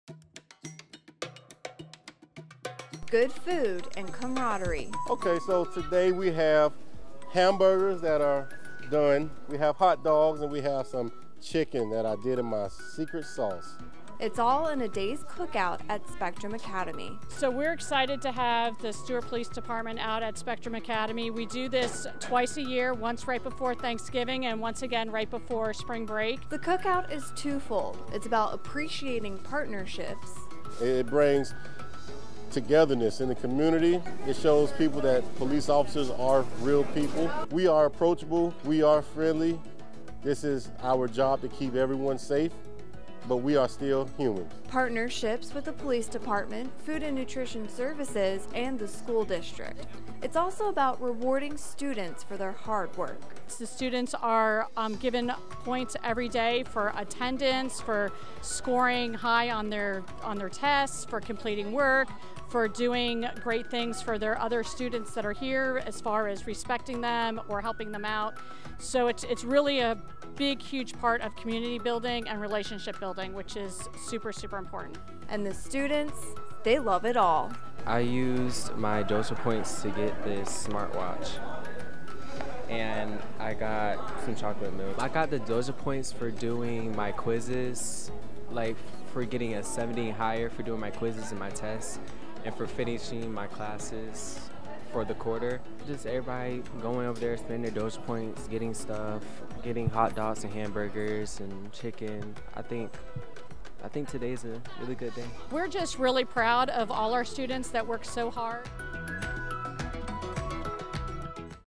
Spectrum Academy Partners With Stuart Police and Hosts Cookout
Twice a year, Spectrum Academy partners with the Stuart Police Department and hosts a delicious cookout for its students.

The cookout not only celebrates the partnerships between Spectrum Academy, Stuart PD and the Martin County School District, but it's also a time to celebrate the hard work of Spectrum students!

Students are rewarded for scoring high on tests, doing good deeds for others, etc. and trade in the points they earned for some awesome prizes, such as a smartwatch or snacks!